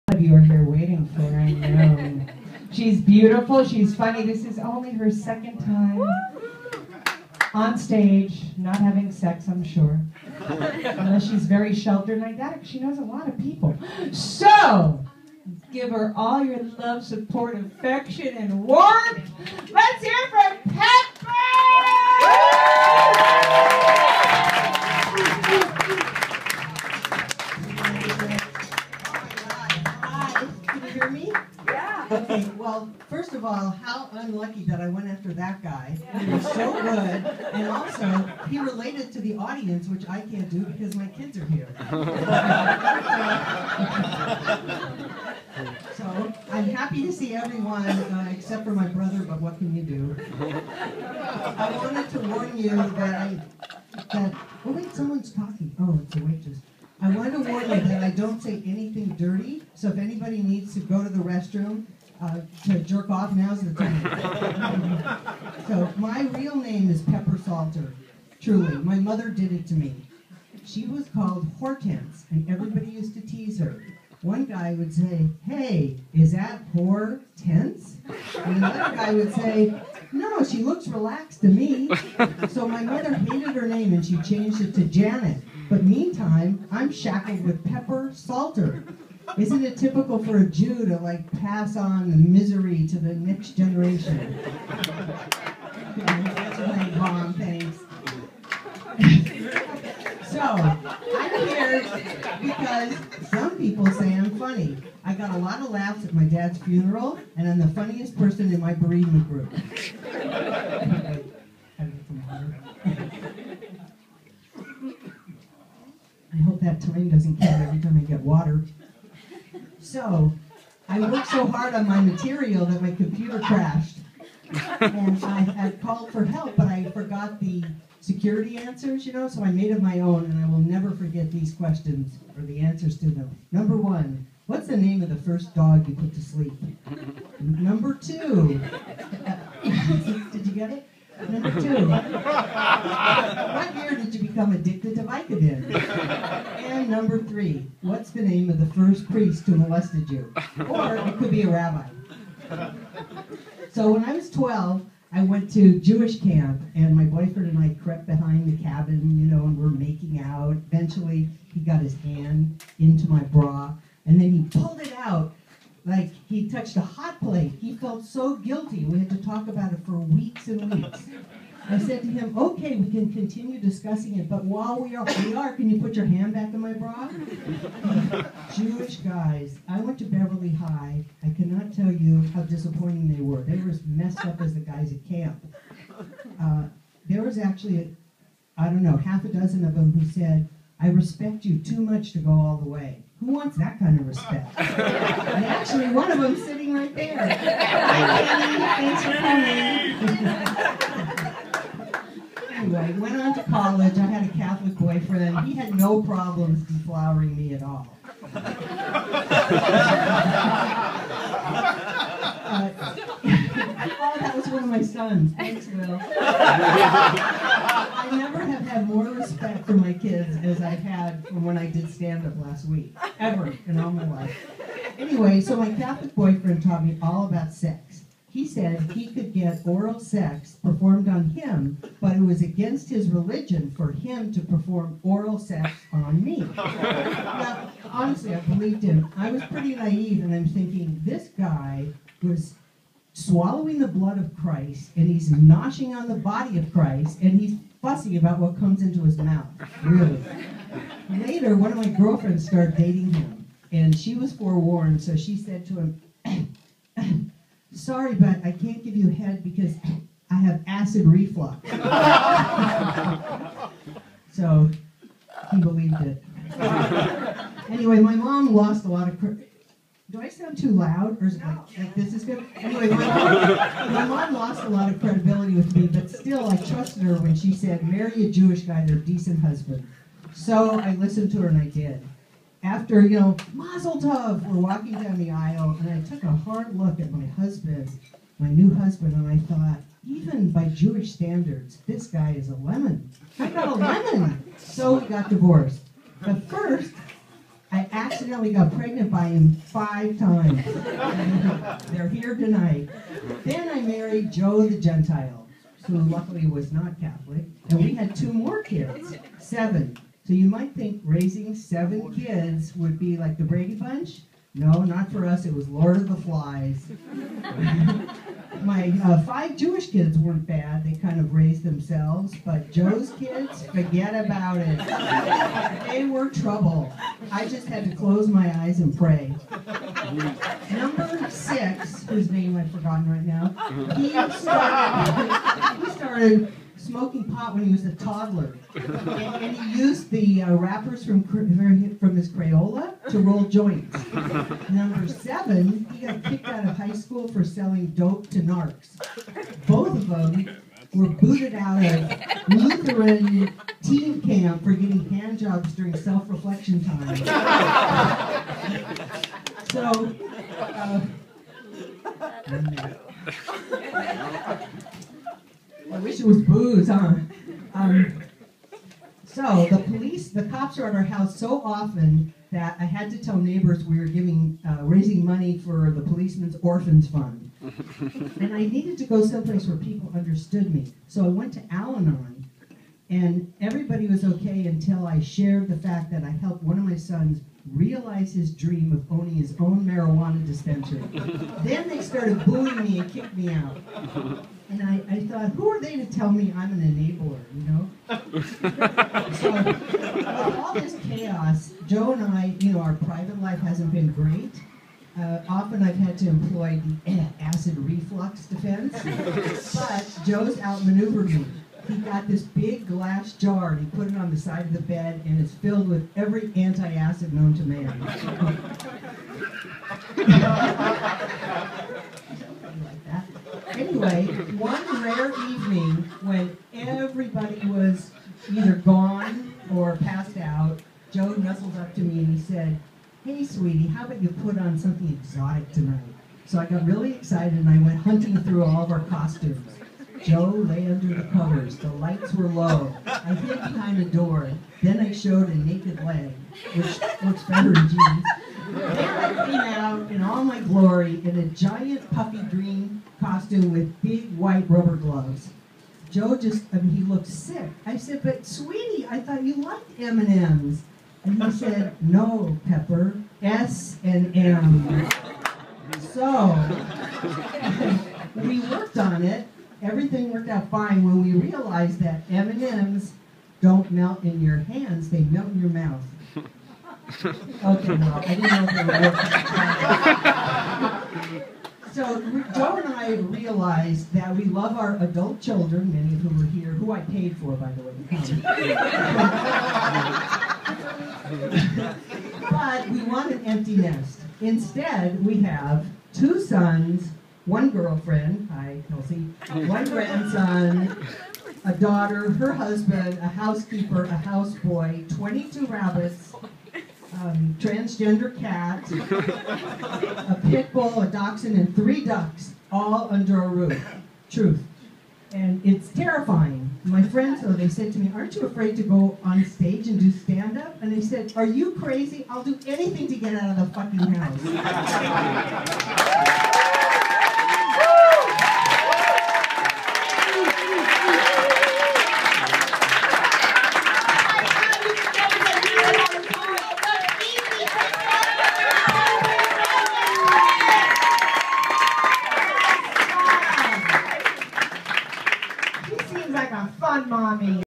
Pepper Salter at The Comedy Store
Pepper Salter at The Comedy Store in Hollywood on 2/27/13